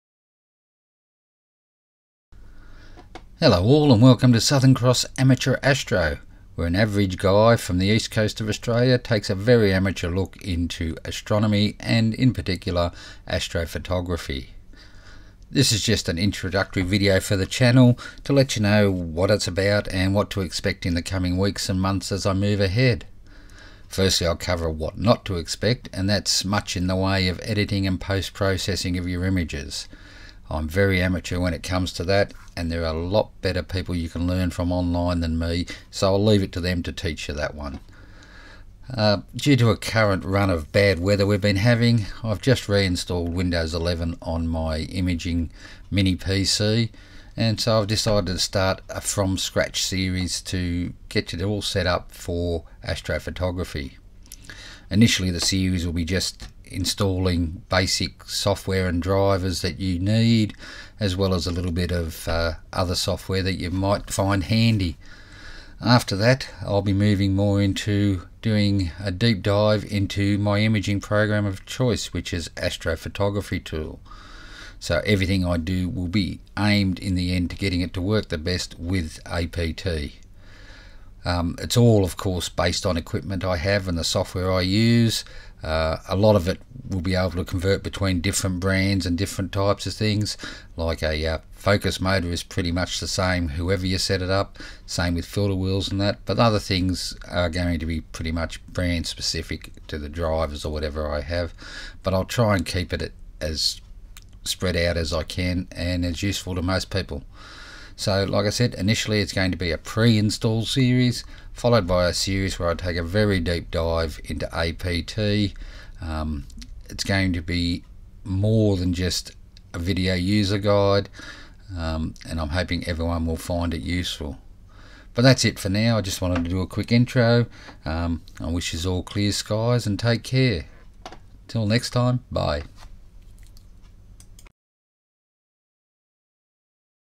SCAA Intro
Welcome all.
Just a quick intro to the channel & the plans for the near future.
I hope viewers will enjoy the channel content & find it useful.